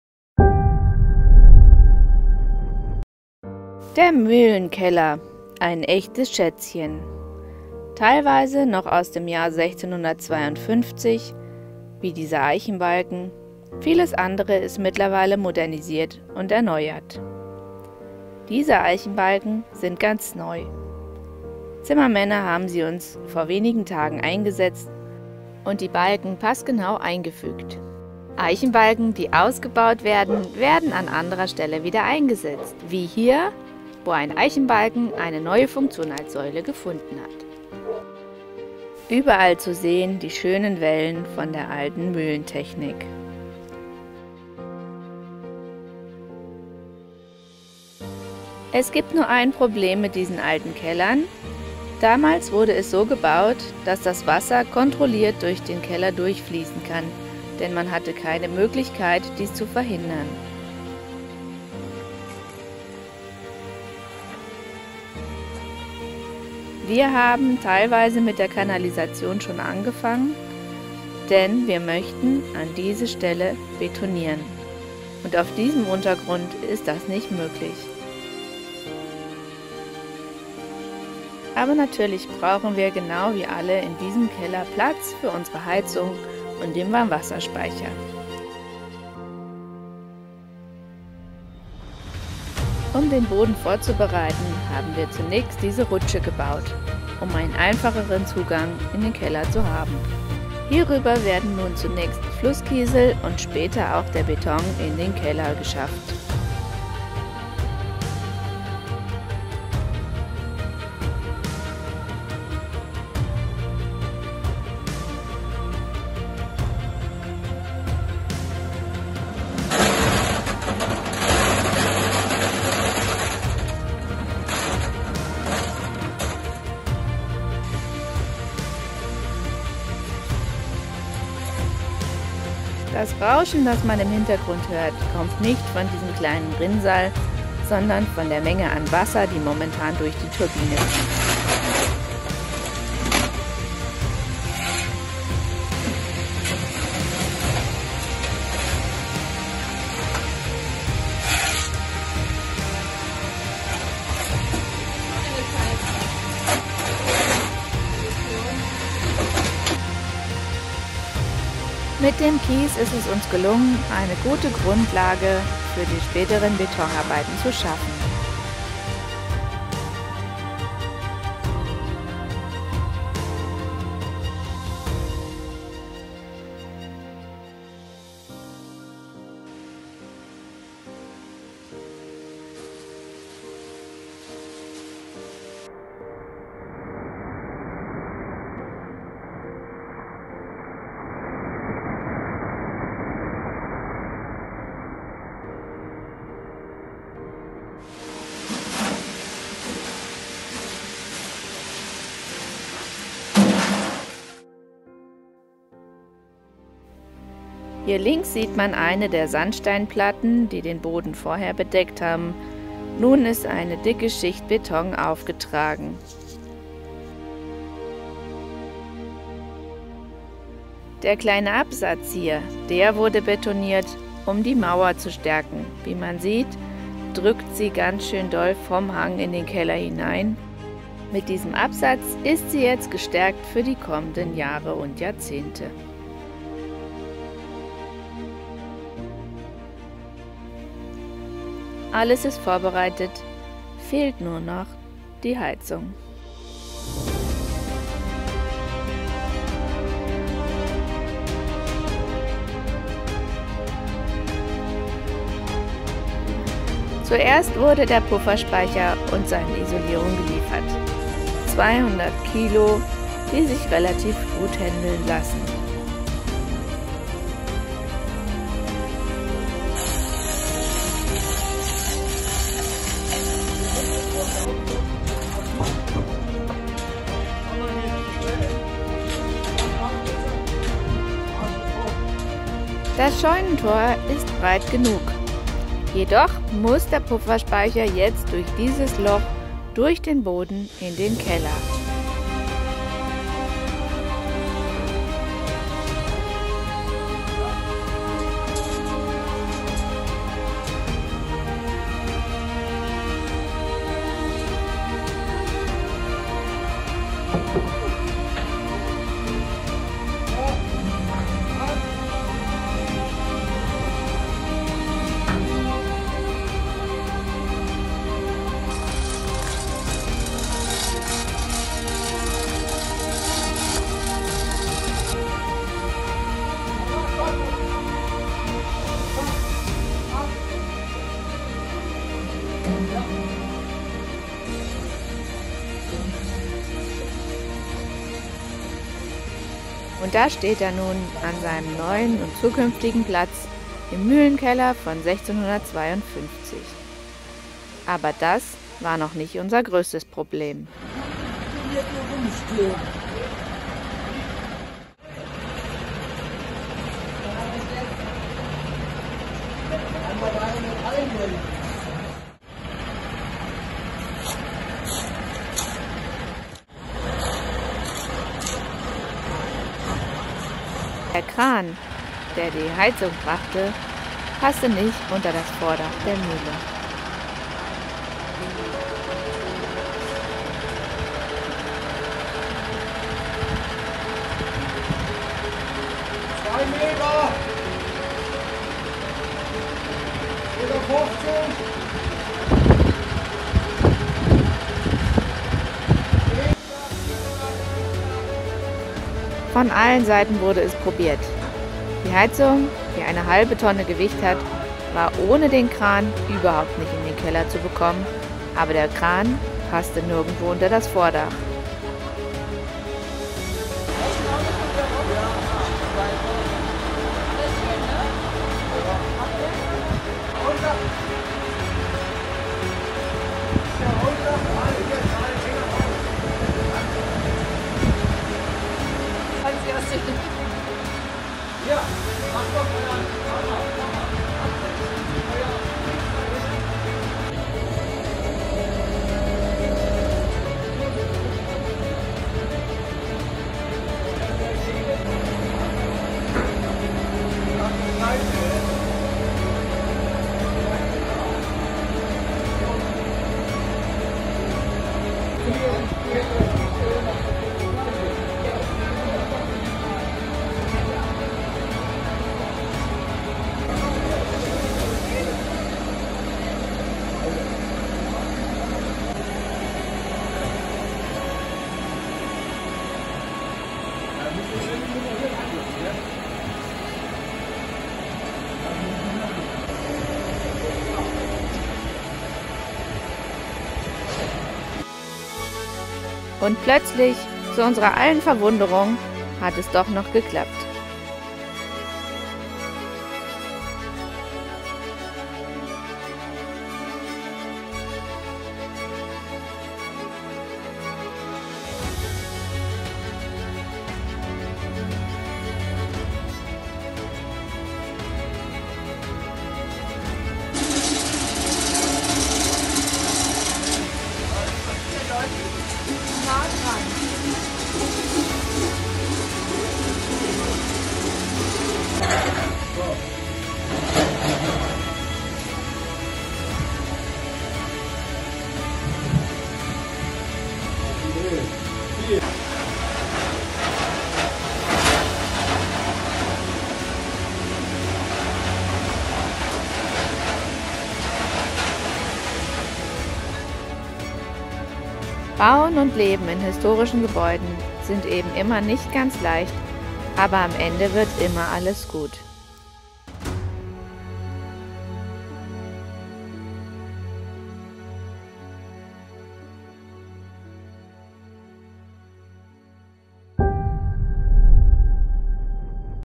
#70 | Alter Keller, neue Technik
In unseren historischen Mühlenkeller von 1652 sollen der Scheitholzkessel und die Pufferspeicher für die Heizung einziehen. Unter dem Sandsteinboden fließt allerdings noch das Hangwasser, was nun durch ein Kiesbett drainiert wird. Anschließend kann betoniert werden.
Dann erst wurde es richtig problematisch: Der Kessel musste durch das alte Mühlentor und mit Unterstützung eines Krans in den Keller gehoben werden. Aber am Ende wird immer alles gut.